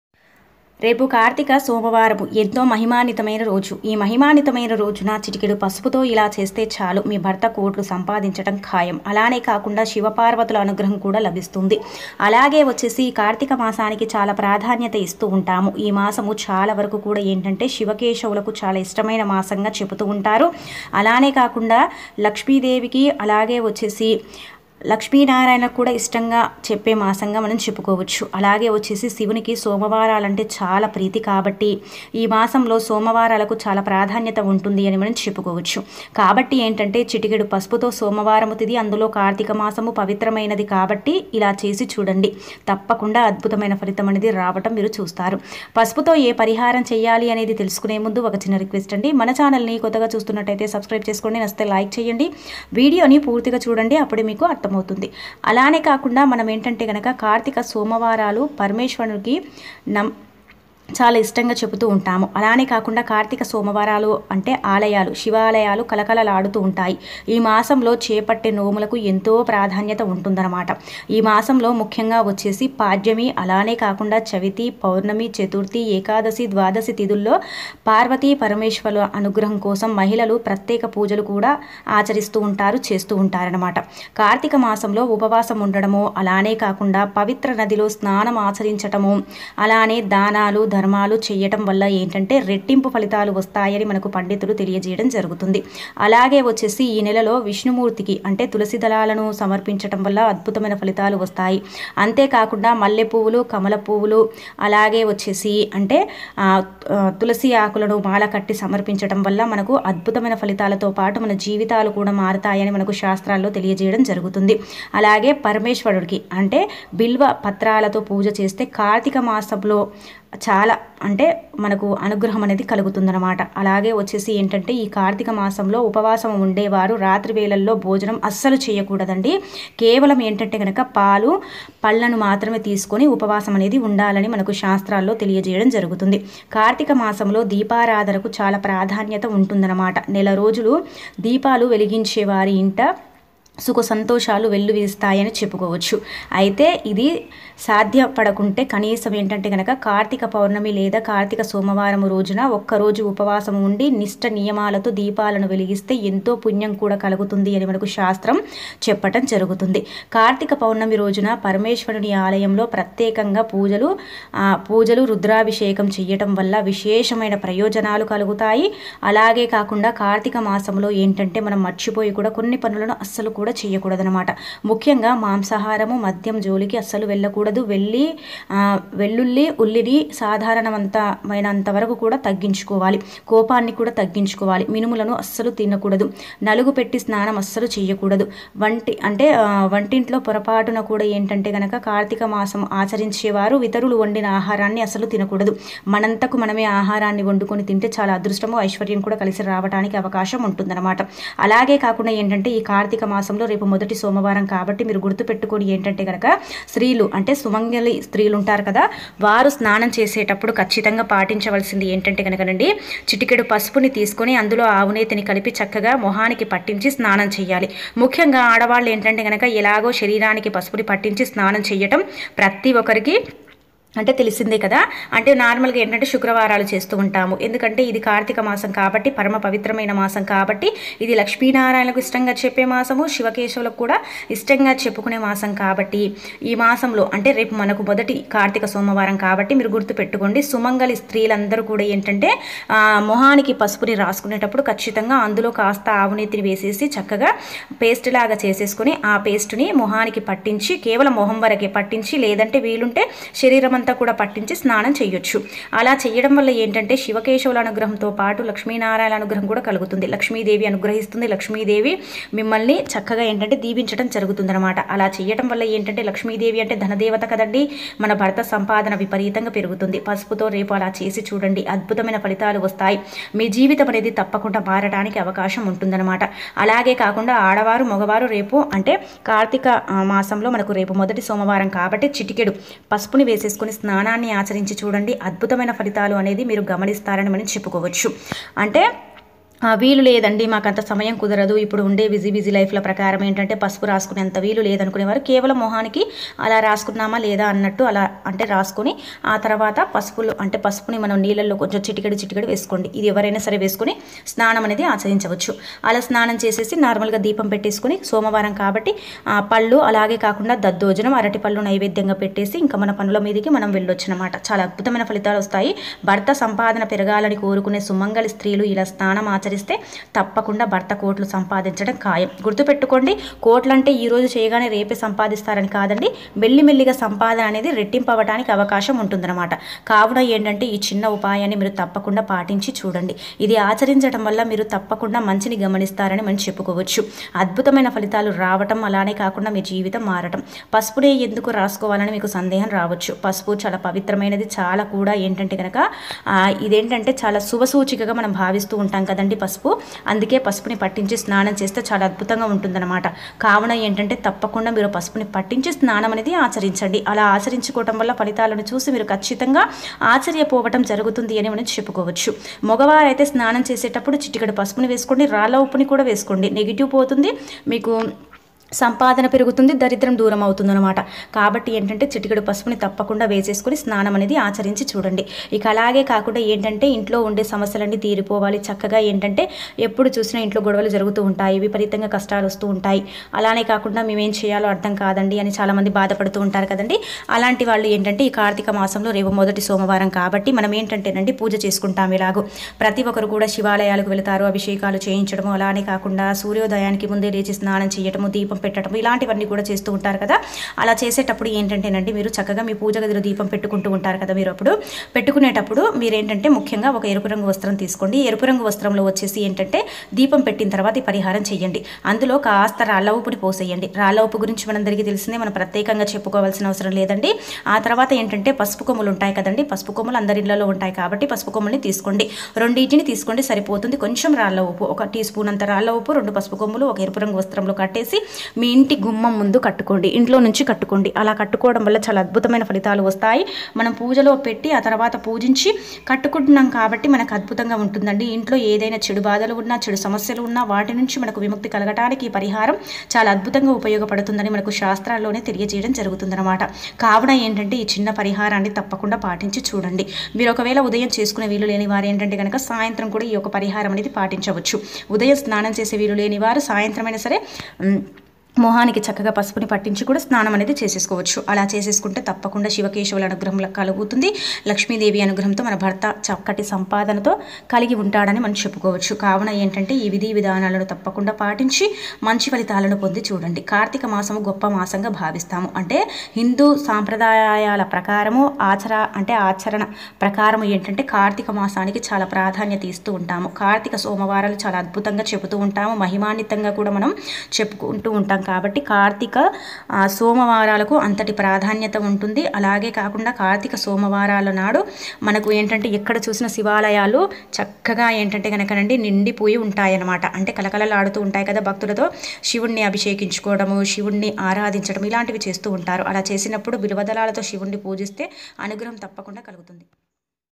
రేపు కార్తిక సోమవారం ఎంతో మహిమాన్వితమైన రోజు రహస్యంగా పసుపుతో ఇలా చేస్తే చాలు భర్త కోట్లు
రేపు కార్తిక సోమవారం ఎంతో మహిమాన్వితమైన రోజు రహస్యంగా పసుపుతో ఇలా చేస్తే చాలు భర్త కోట్లు సంపాదించడం కాదు